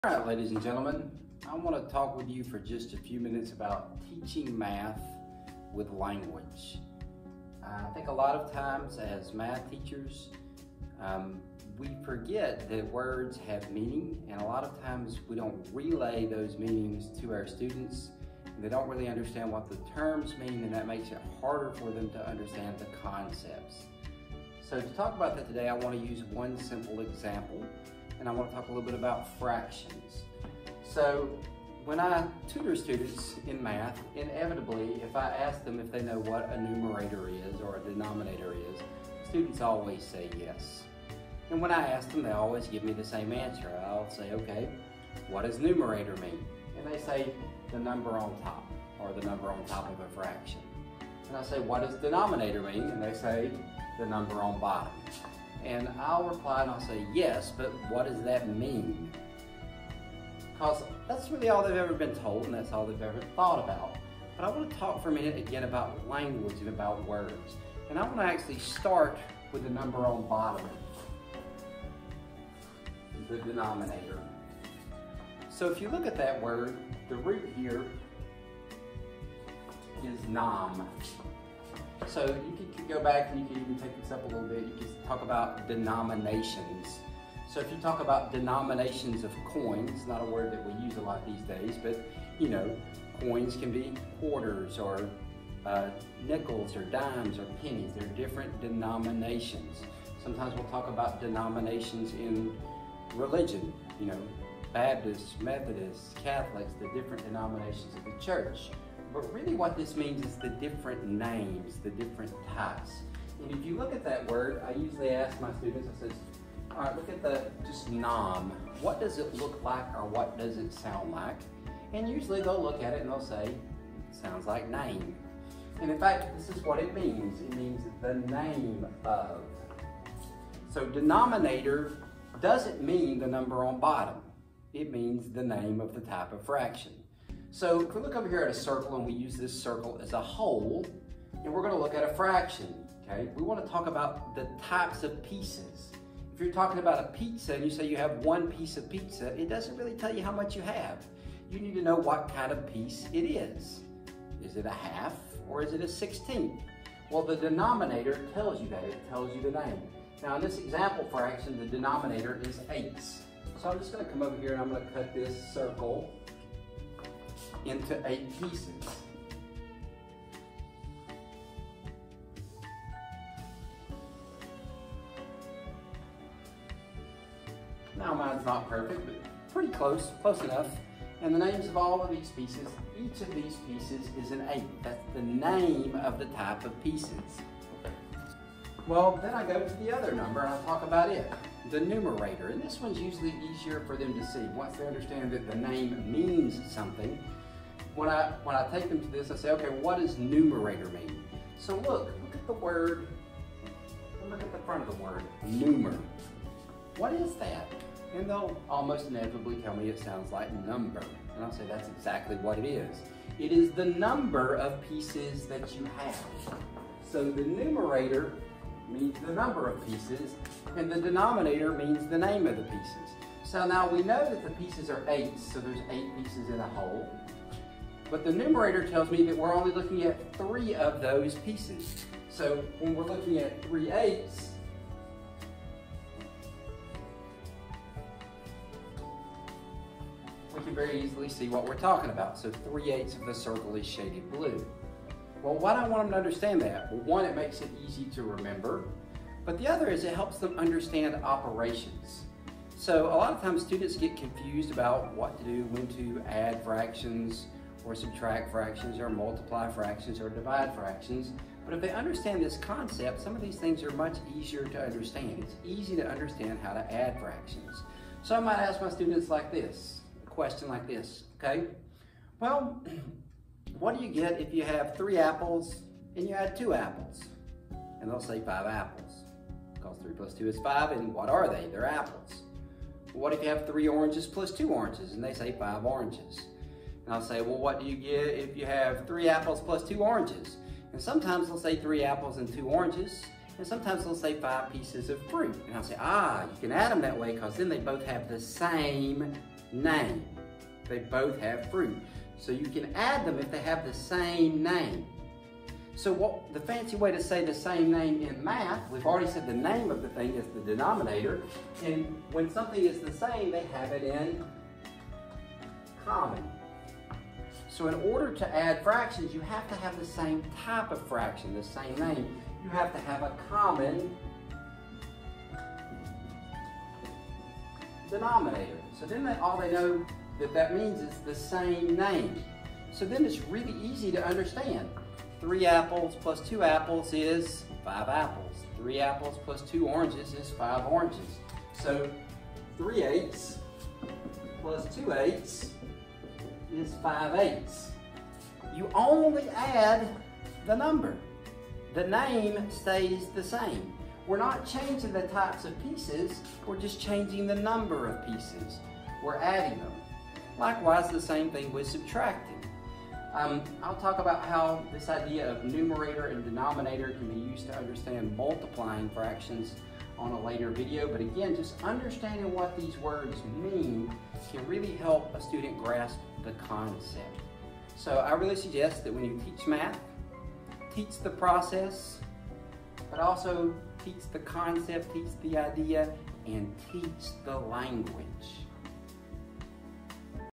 Numerator and Denominator: Teaching Math with Language
This video explains how teaching language and root words can clarify mathematical concepts.  Numerator and denominator are used as example.